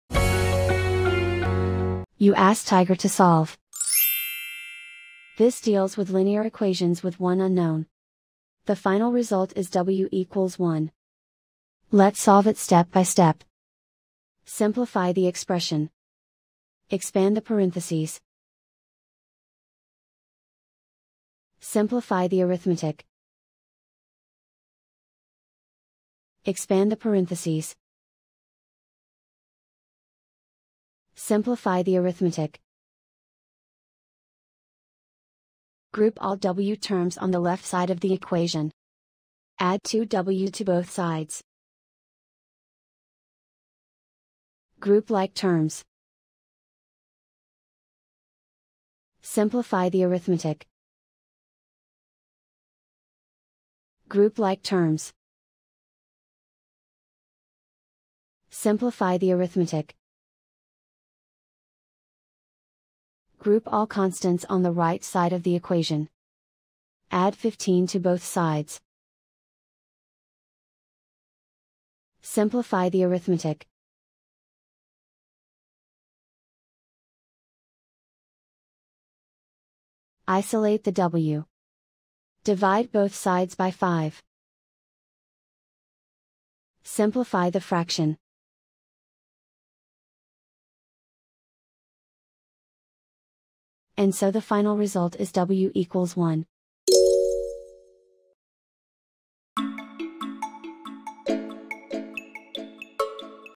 Solve 3(w-5)=-2(w+5): Linear Equation Video Solution | Tiger Algebra
Struggling with 3(w-5)=-2(w+5)? Watch our step-by-step video on Tiger Algebra to master this linear equation effortlessly. Also check out for more tips and tricks for dealing with linear equations with one unknown